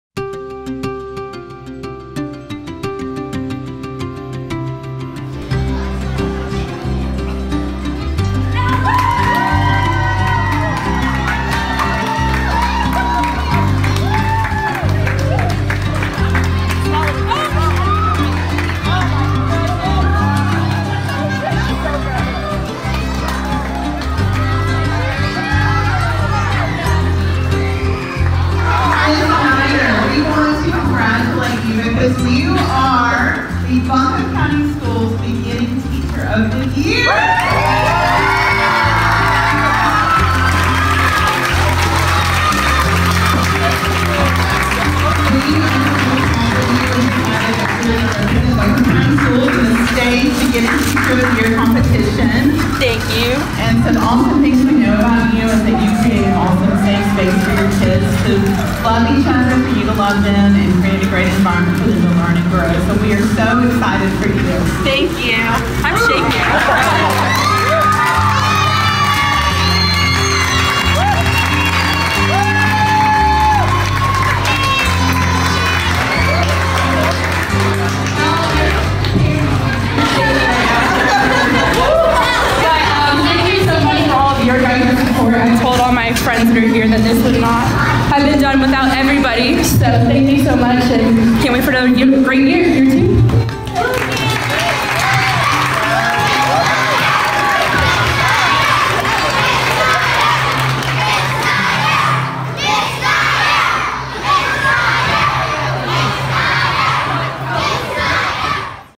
The Inaugural BCS Beginning Teacher of the Year
Weaverville Elementary School’s Allie Snyder has been named the inaugural Buncombe County Schools Beginning Teacher of the Year! Ms. Snyder was surprised with the news in her school’s cafeteria today by teachers, friends, and students waiting for her to walk in! 🎥 Here’s our BCSlive video of the big moment.

Snyder’s teaching philosophy centers around viewing students and staff as whole individuals. She believes that success grows from being valued as an individual and being challenged beyond your comfort zone. She sees her students as whole people and works to make lessons relevant to them.

“Yes, we focus on academics, but our students also love soccer and they love Legos. It’s important to tie their hobbies and interests into teaching,” said Ms. Snyder

The Beginning Teacher of the Year (BTOY) Program began this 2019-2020 school year through NCCAT-The North Carolina Center for the Advancement of Teaching. The award program is designed to honor and retain excellent North Carolina teachers and education leaders among the 7,000 beginning teachers recruited annually.

Congratulations to this wonderful teacher with a bright future!